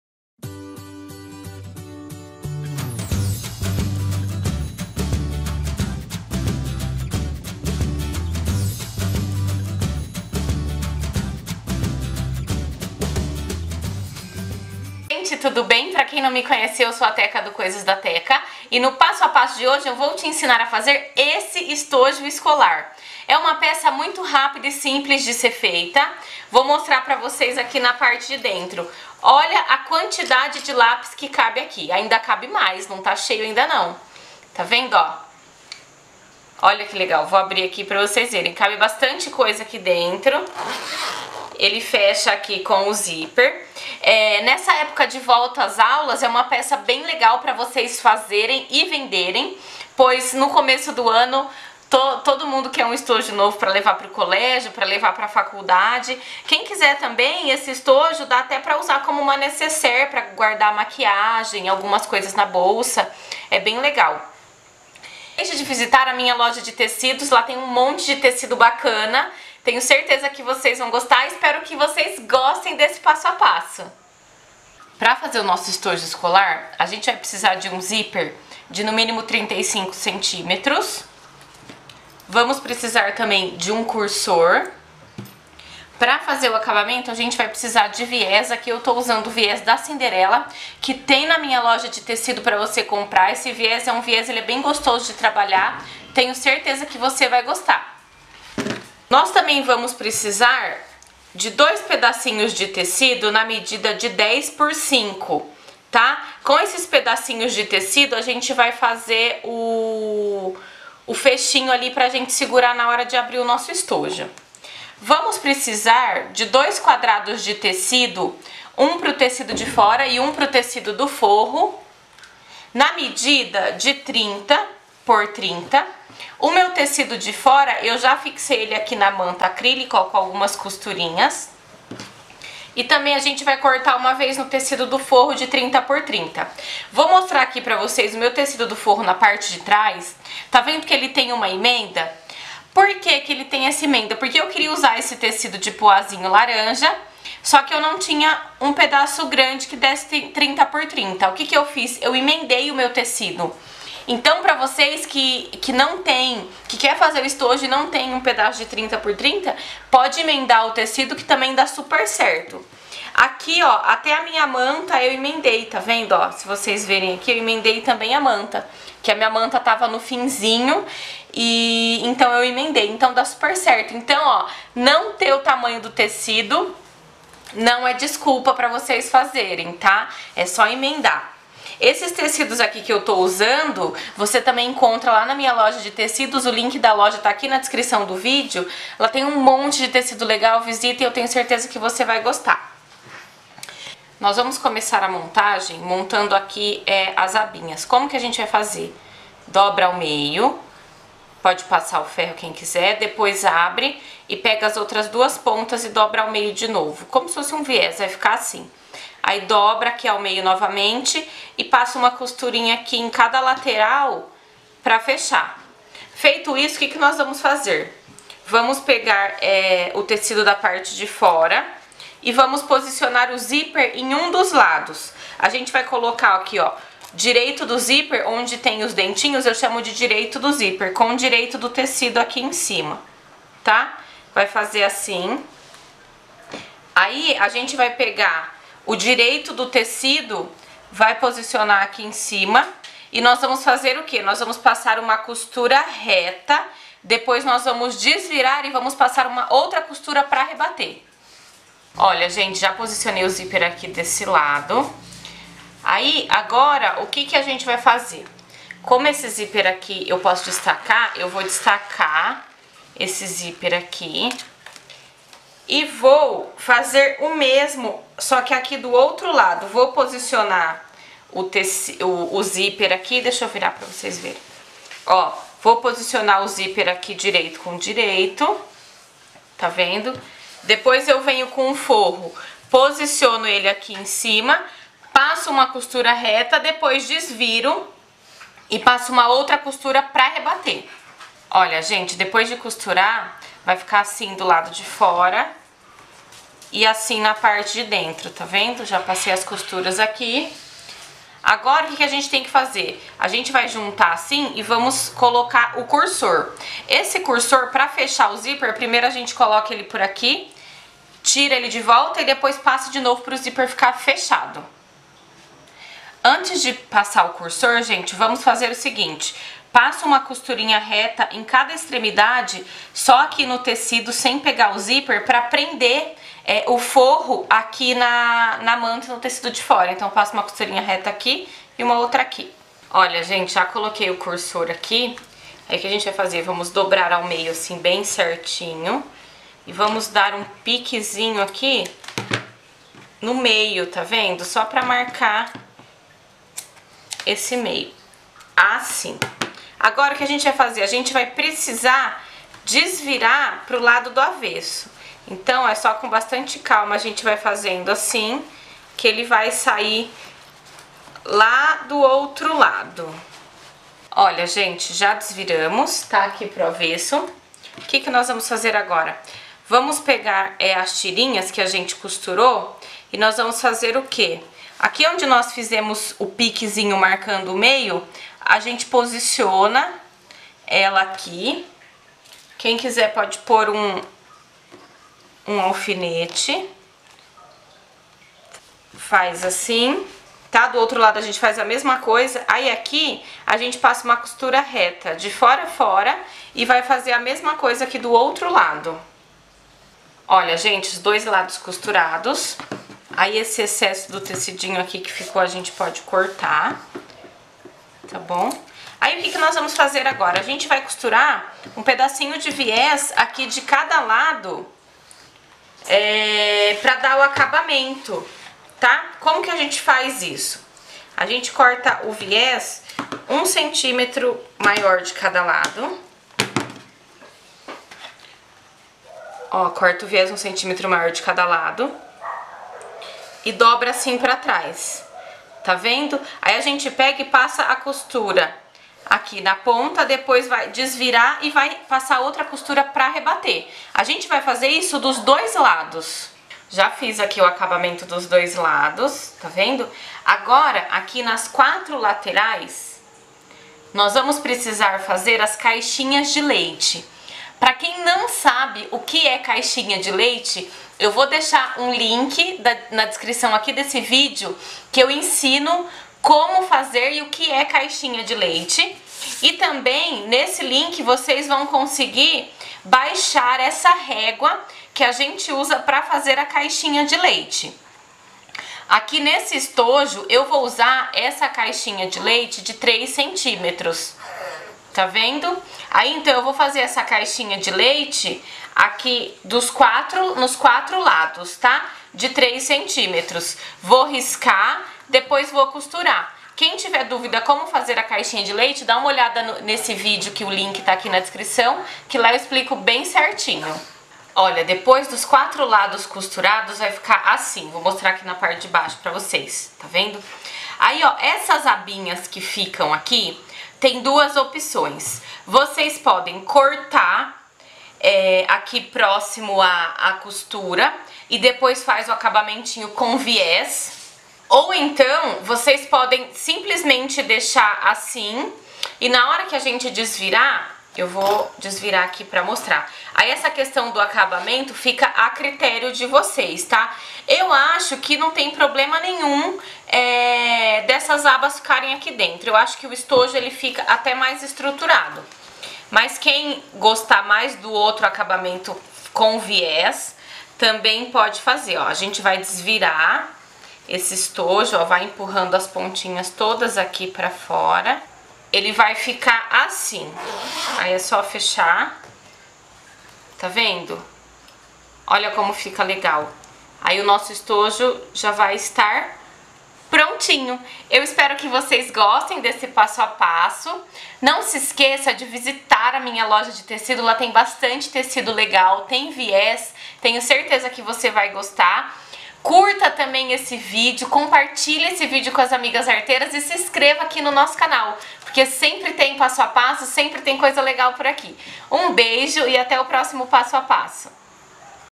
DIY - Passo a Passo Estojo Escolar
No vídeo de hoje vou te ensinar como fazer um estojo escolar! Nessa época de volta as aulas, o estojo escolar é uma peça super vendável!

Todos os tecidos usados nesse passo a passo você encontra em minha loja de tecidos!